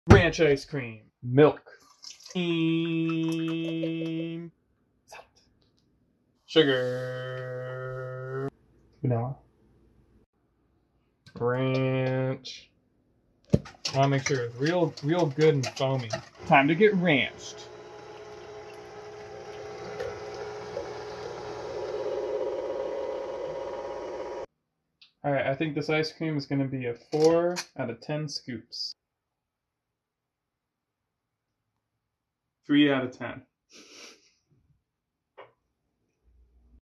Ranch Ice Cream
Making Ranch Ice Cream
See how to make this homemade Ranch Ice Cream! 300% of your daily calories in one serving

Recipe
1 Cup Milk
2 Cups Heavy Cream
1 Cup Sugur
2 Ranch